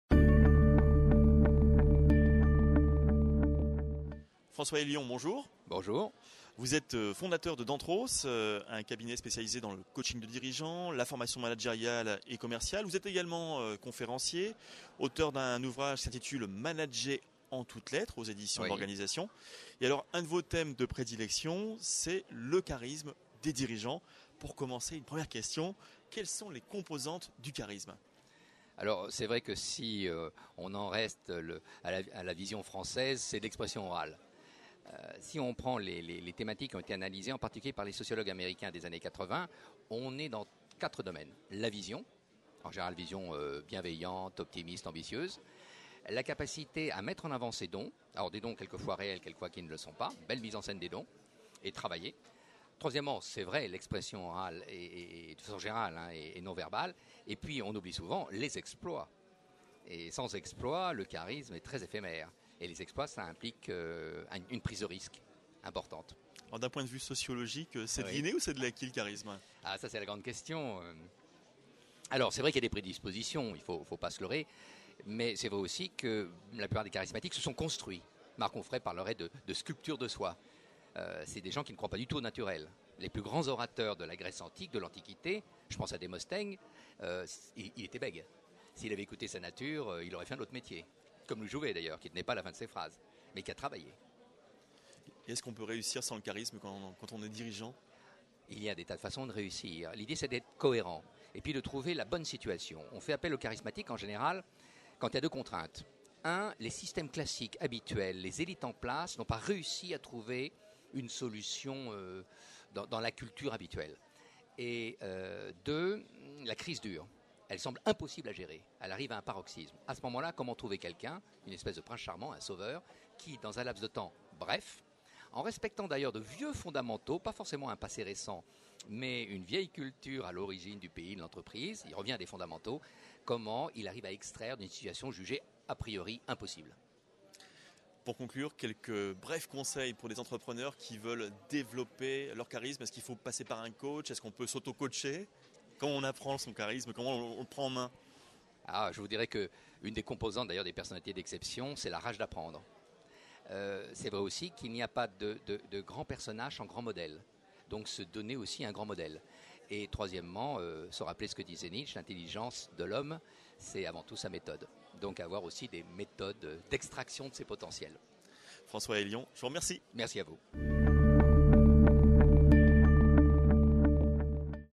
Comment et pourquoi développer son charisme ?
François Aélion, auteur et Fondateur de Danthros, donne ses conseils aux entrepreneurs qui souhaitent développer leur charisme.